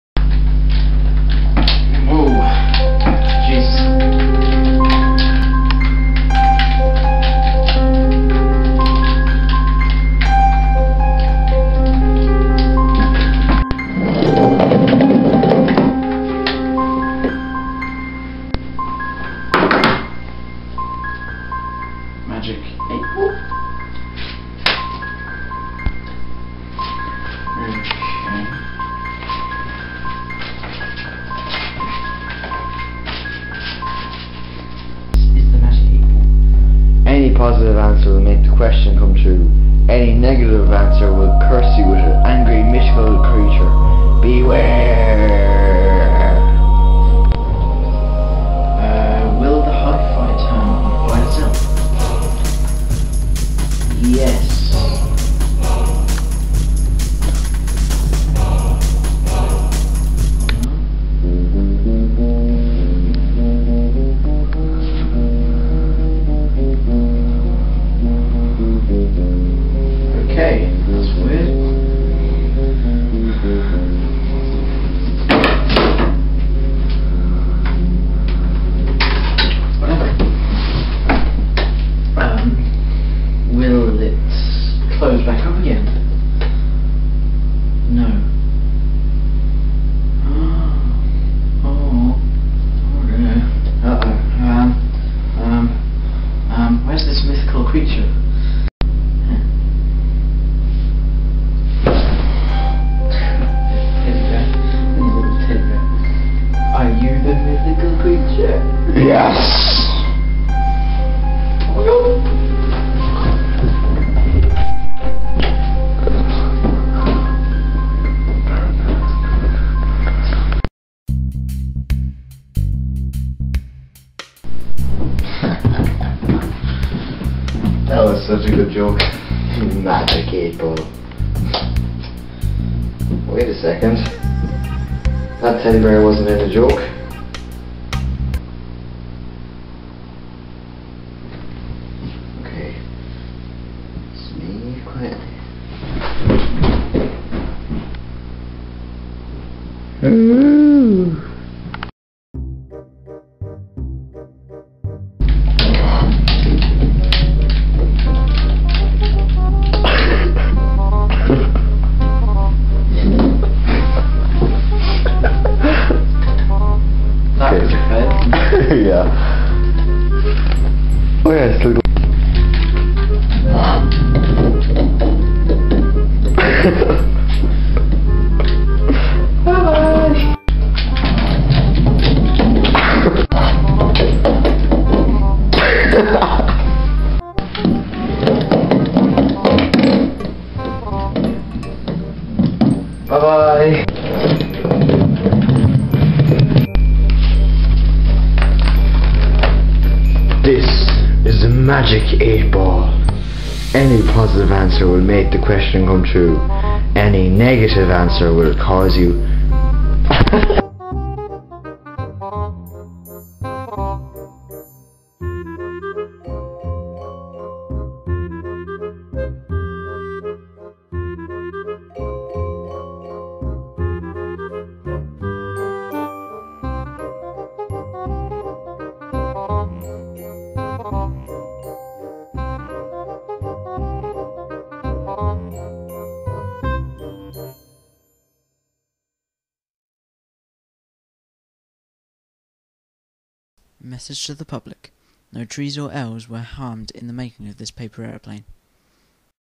The "MAG!C" 8 Ball !!!
This is NoteBook Films second Video. Its basically a thing me and darragh thought up on the spot and it came to a short comedy. hope you guys and gals enjoy it.